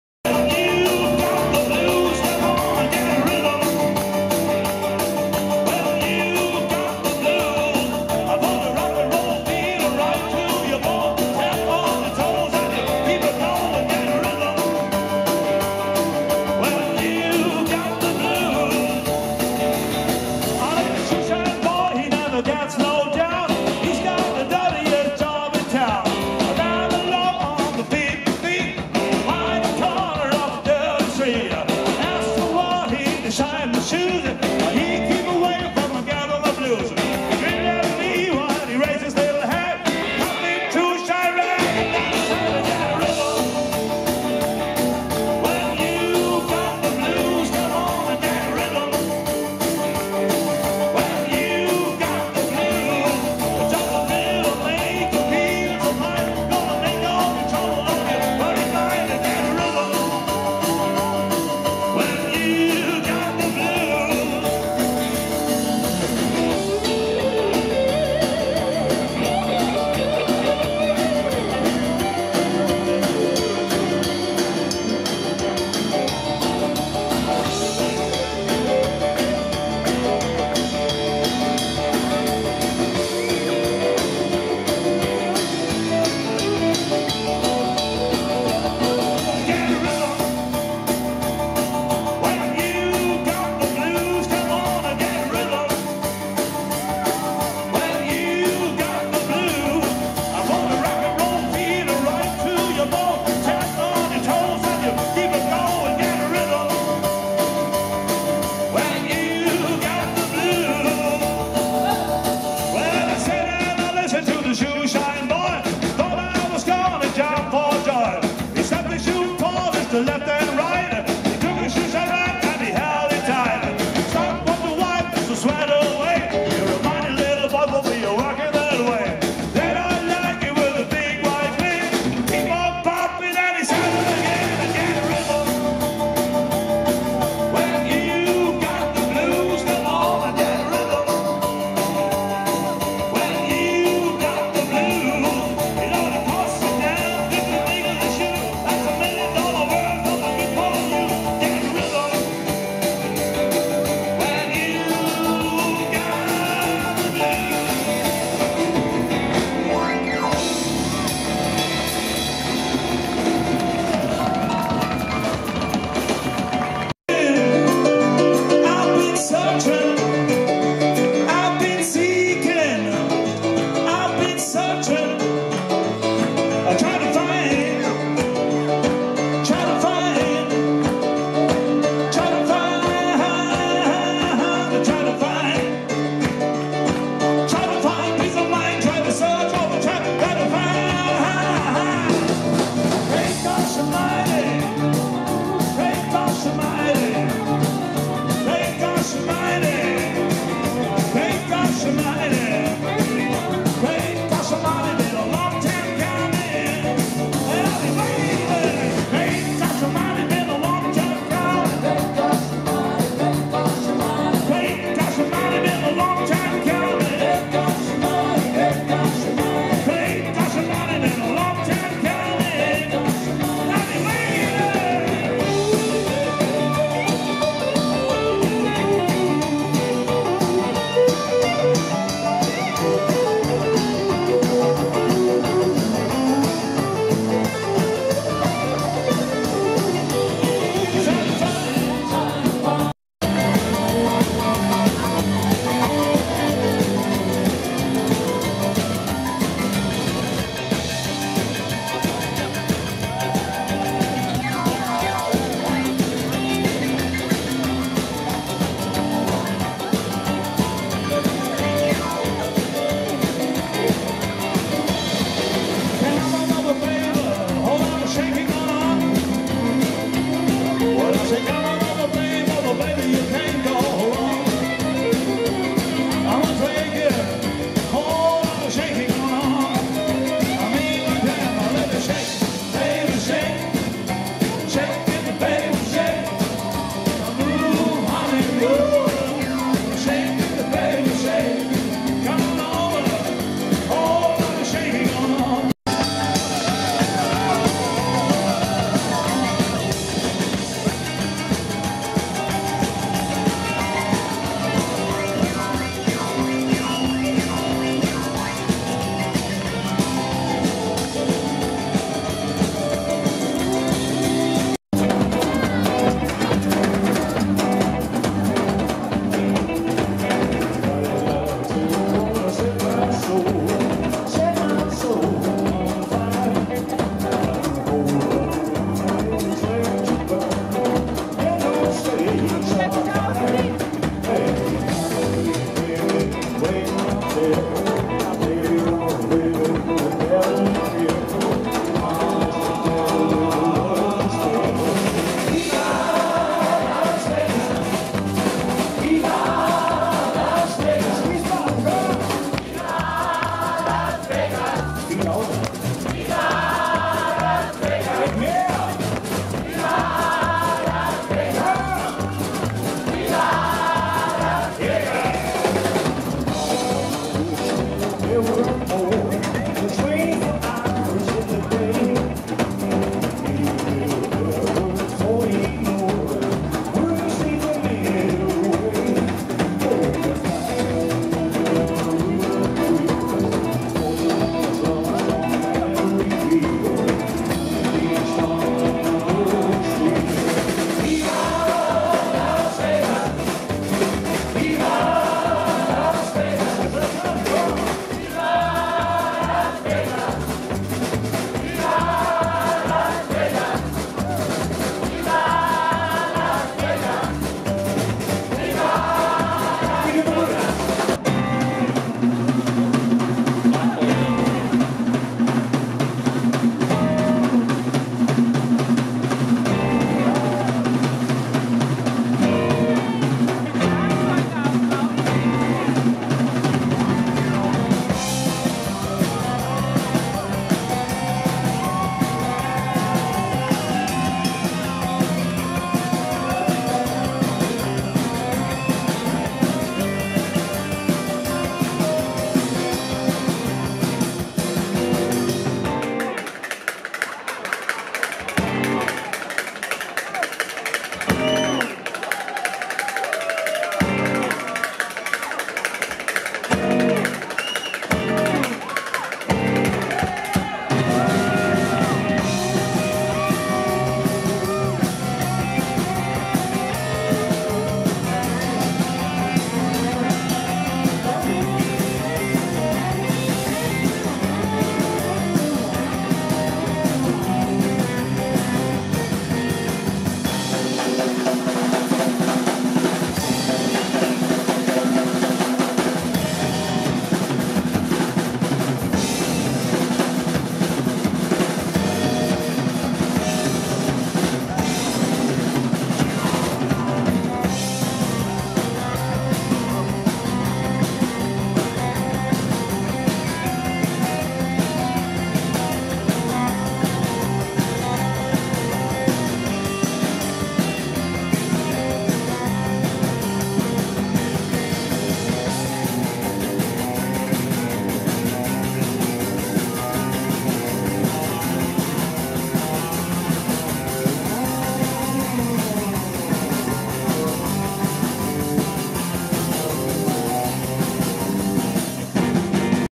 Krüger Rockt - Weinfest Wiesbaden 2015 (2)
Joachim Villwock - Guitar
Patrick Daniel - Bass
Walt Bender - Drums
Harald Krüger - Piano
Für Krüger Rockt! lohnt sich immer auch eine etwas weitere Anfahrt

aufgenommen von Mapixx